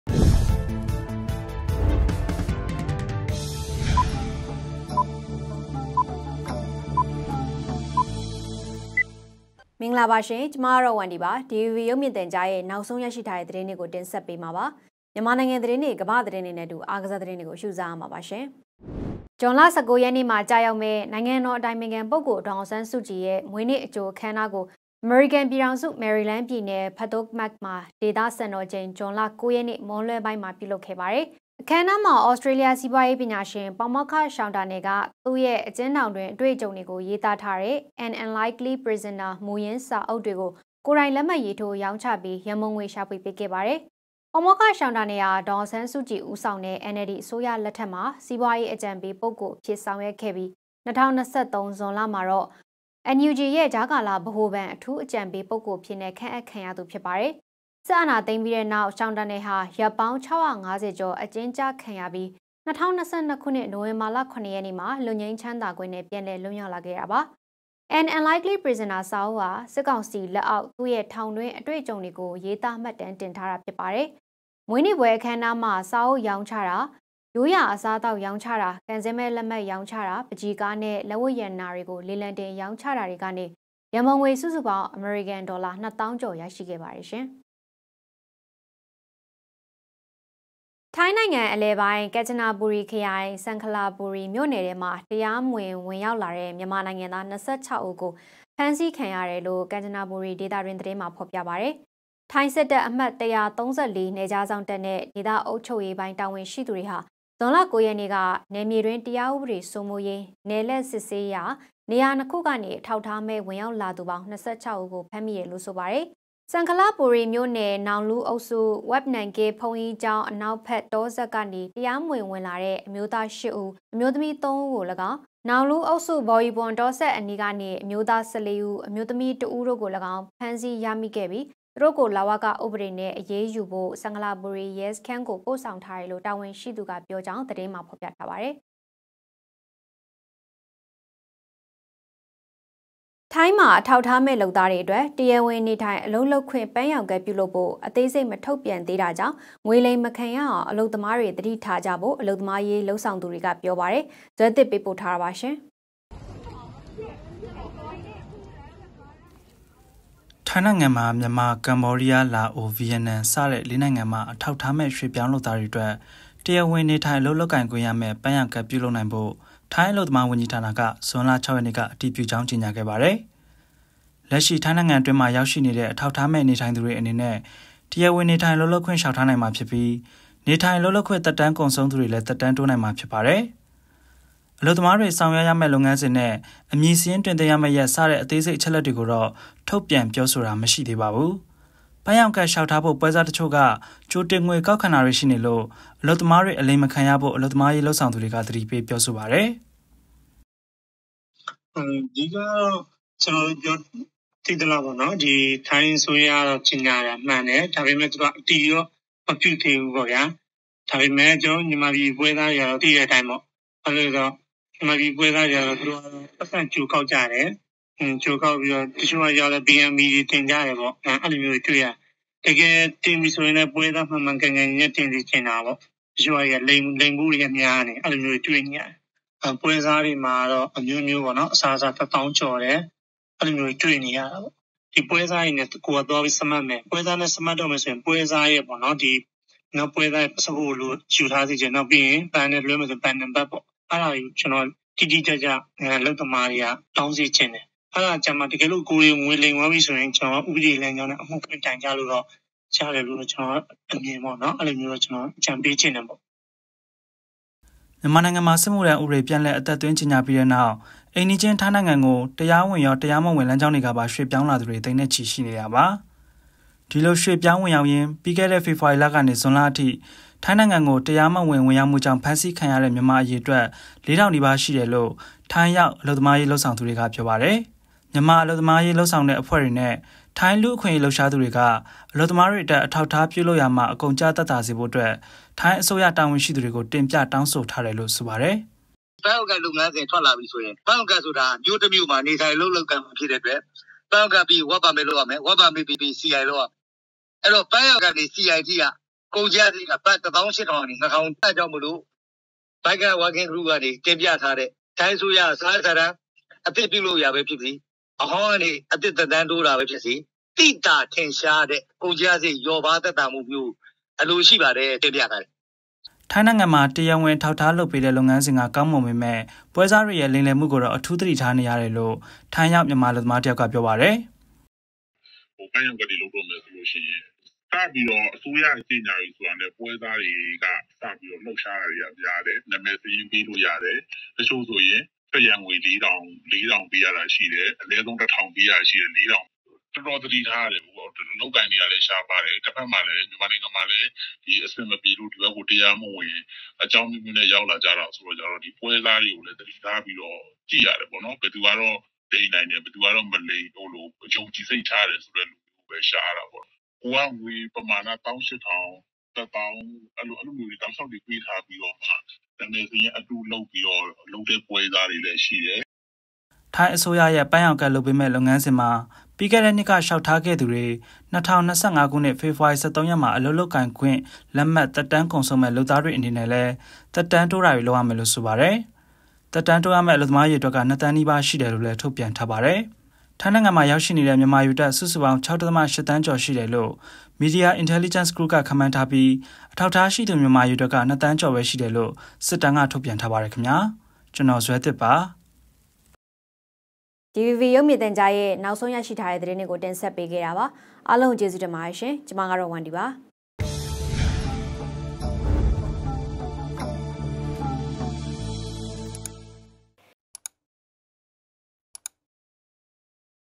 DVB Digital နေ့လယ် ၁ နာရီ သတင်း (၁၀ ရက် ဇွန်လ ၂၀၂၄)
- မယ်ရီလန်းပြည်နယ်မှာ ဒေါ်အောင်ဆန်းစုကြည်ရဲ့မွေးနေ့အခမ်းအနားကျင်းပ။
- ကန်ကျနပူရီမှာ မြန်မာ ၂၆ ဦး ဖမ်းဆီးခံရ။